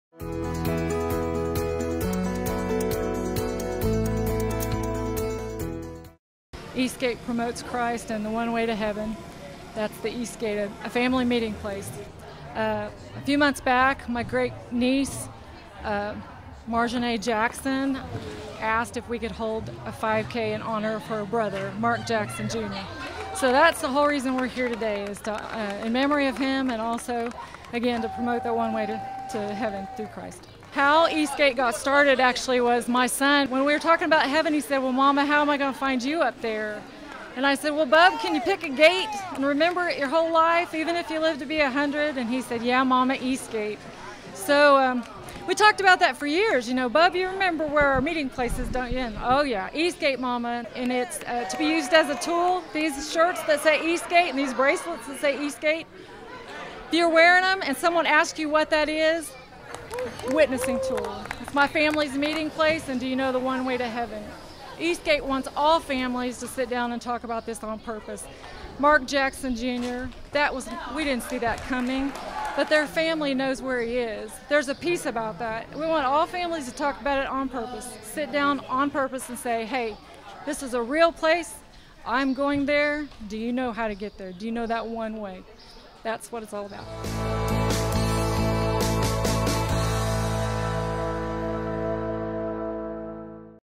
Decatur East Gate 5K
Today the East Gate Foundation held their inaugural 5K run in honor of Mark Jackson Jr. of Decatur.